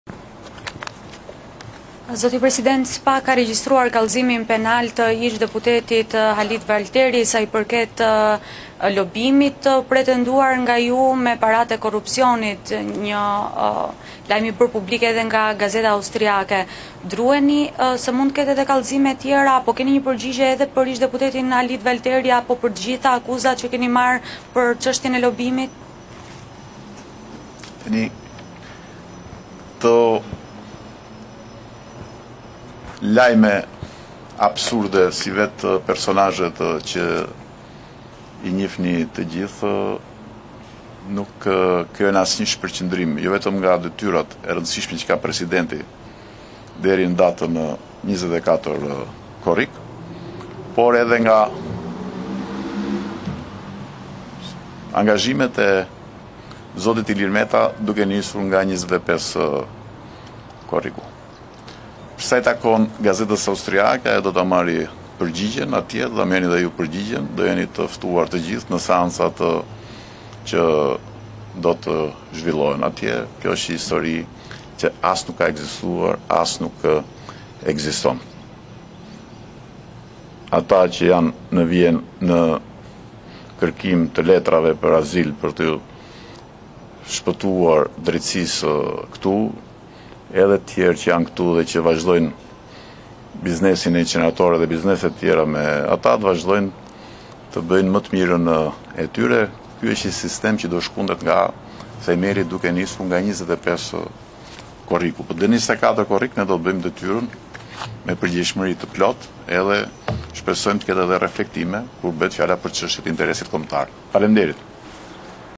Presidenti Ilir Meta i përgjigjet akuzave të ish-deputetit Halit Valteri
I pyetur nga gazetarët nëse i druhet kallëzimeve të tjera në SPAK si dhe nëse ka përgjigje për  artikullin e gazetës austriake për një lobim mijëra euro, Meta tha se këto lajme janë absurde si vetë personazhet dhe se kjo është një histori që as nuk ka ekzistuar dhe as nuk ekziston.

Ai tha se sistemi do të shkundet në themel që nga 25 korriku. Ilir Meta: Këto lajme absurde si vetë personazhet që i njihni të gjithë nuk krijojnë asnjë shpërqëndrim jo vetëm nga detyrat që ka presidenti deri në datën 24 korrik por edhe nga angazhimet e zotit Ilir Meta duke nisur nga 25 korriku. Sa i takon gazetës austriake do ta marrë përgjigjen në seancat që do të zhvillohen atje. Është një histroi që nuk ka ekzistuar dhe as nuk ekziston. Ata që janë në kërkim për azil për ti shpëtuar drejtësisë këtu dhe të tjerë që janë këtu e që vazhdojnë biznese me ta të bëjnë më të mirën e tyre. Ky është një sistem që do të shkundet në themel që nga 25 korriku. Deri më 24 korrik ne do të bëjmë deyrën me përgjegjësi të plotë dhe shpresojmë të ketë dhe reflektime kur bëhet fjala për çështje të interesit kombëtar.